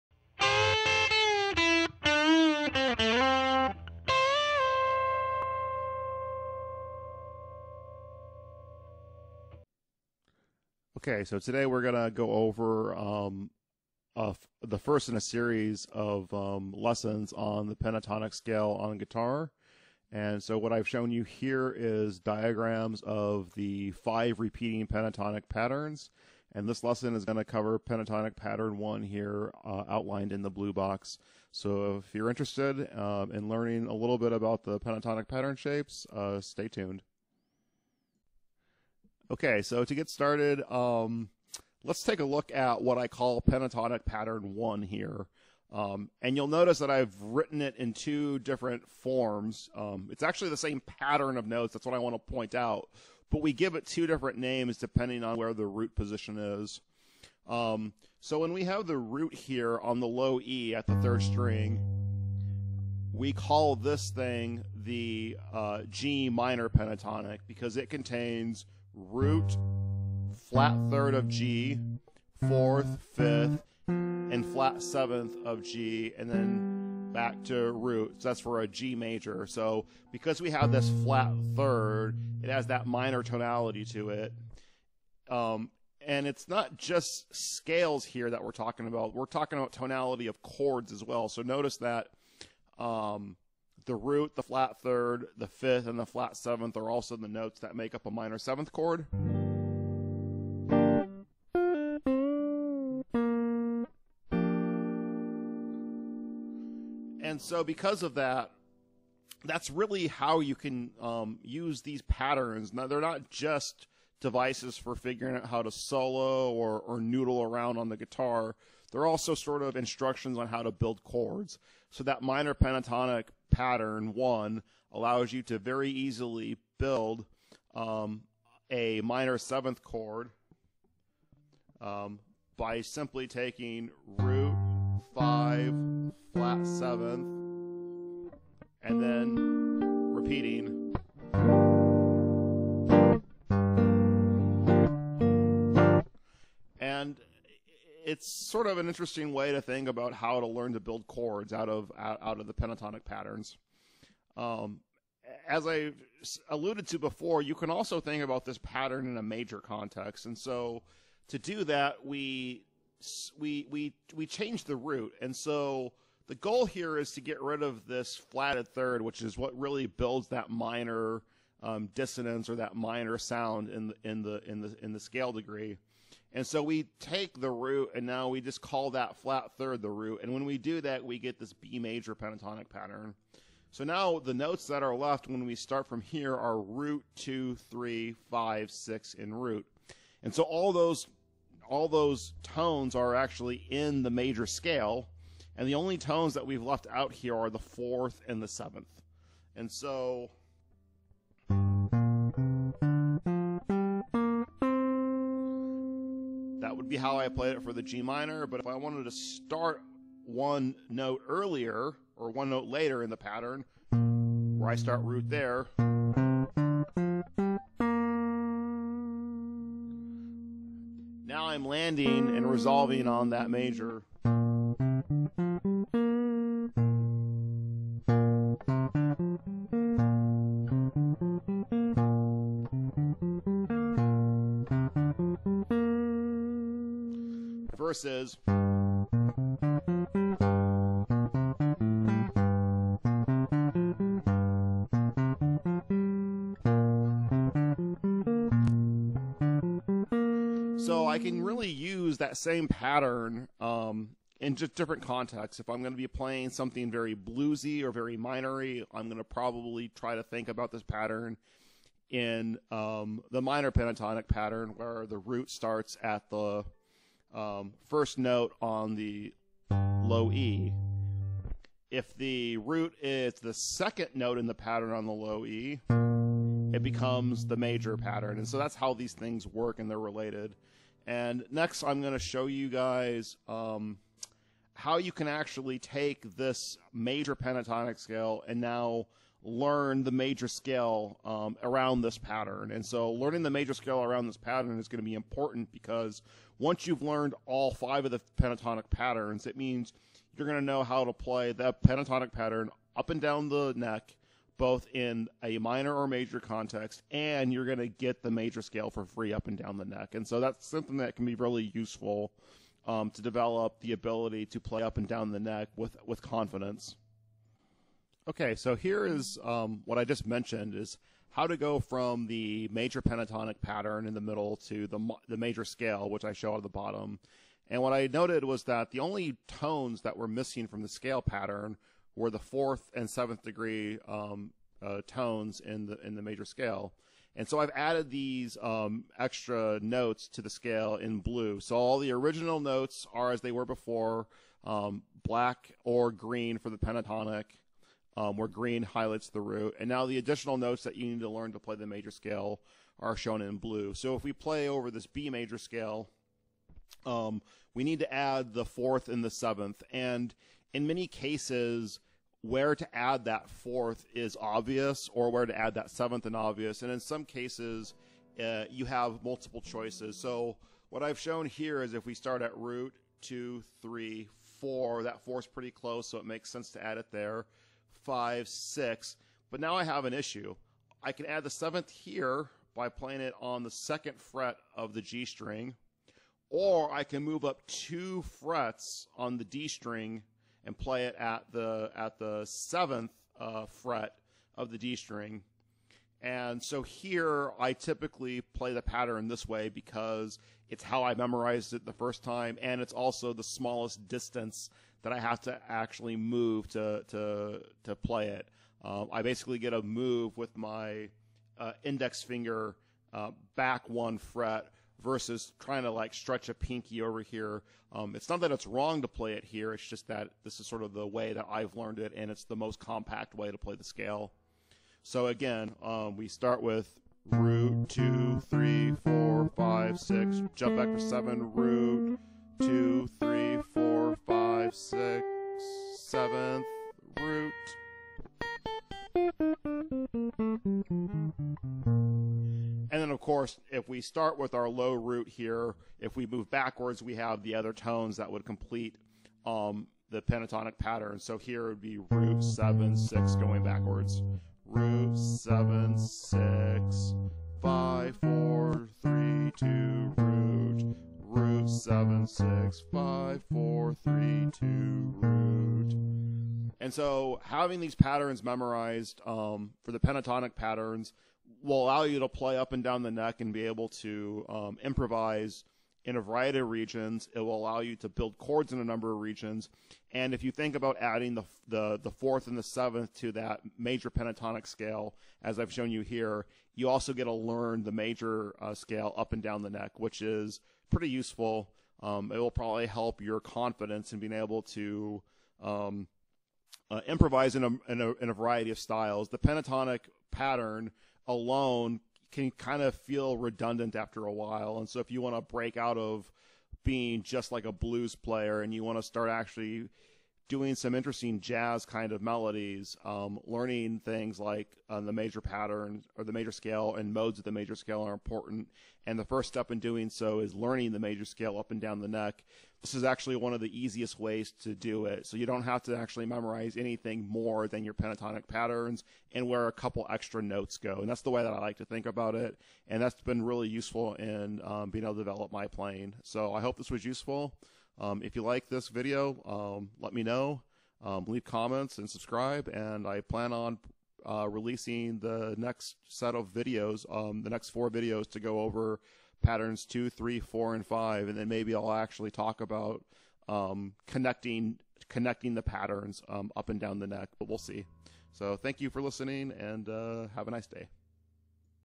Guitar: Pentatonic Pattern 1 + Maj Scale
A Lesson on Pentatonic Scales, Min / Maj Pentatonic + Major Scale around Pentatonic  Pattern 1